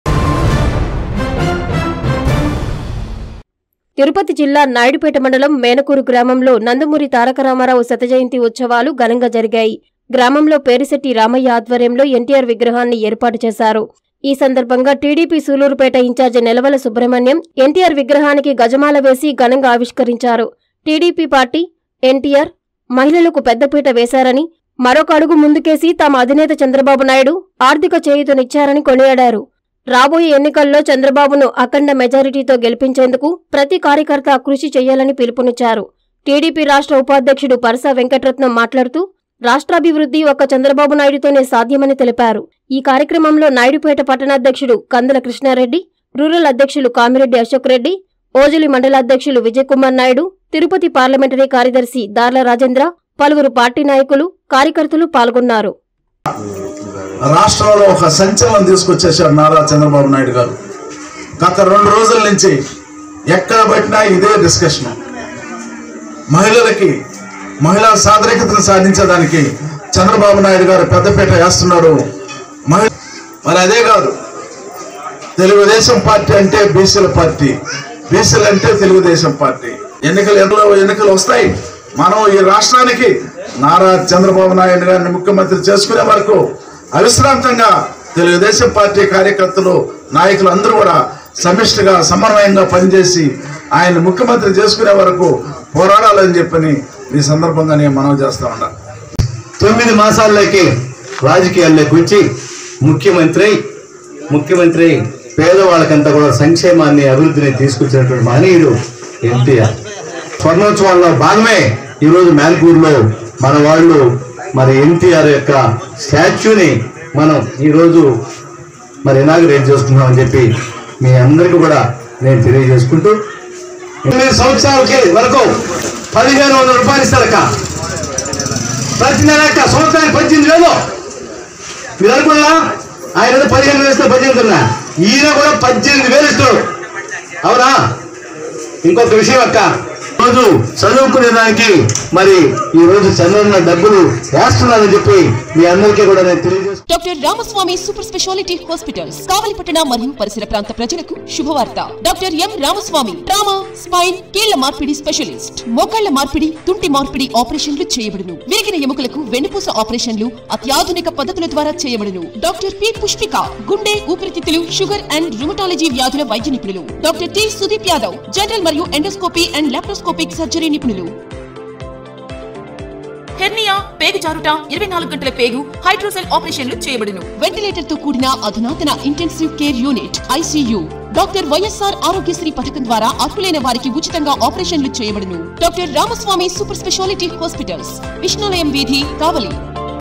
నాయుడుపేట‌లో ఘ‌నంగా ఎన్టీఆర్ శ‌త జ‌యంతి ఉత్స‌వాలు...
- పేరిశెట్టి రామ‌య్య ఆధ్వ‌ర్యంలో దివంగ‌త రామారావు విగ్ర‌హం ఏర్పాటు
- పాల్గొన్న సూళ్లూరుపేట ఇన్‌చార్జి నెల‌వ‌ల